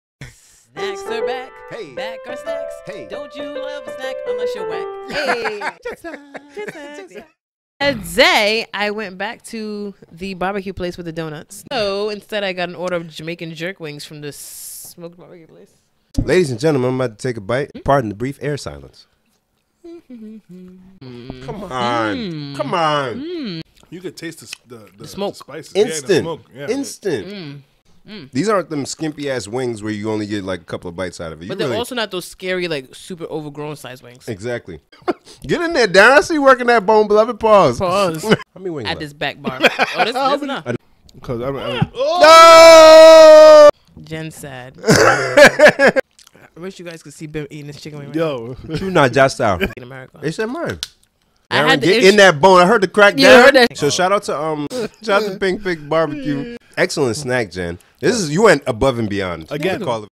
Please Delete This - Episode 14 - Gens Snacks
On episode 14s Gens Snacks segment Gen blessed us with delicious Jamaican Jerk wings from Pink Pig BBQ in Warwick Rhode Island.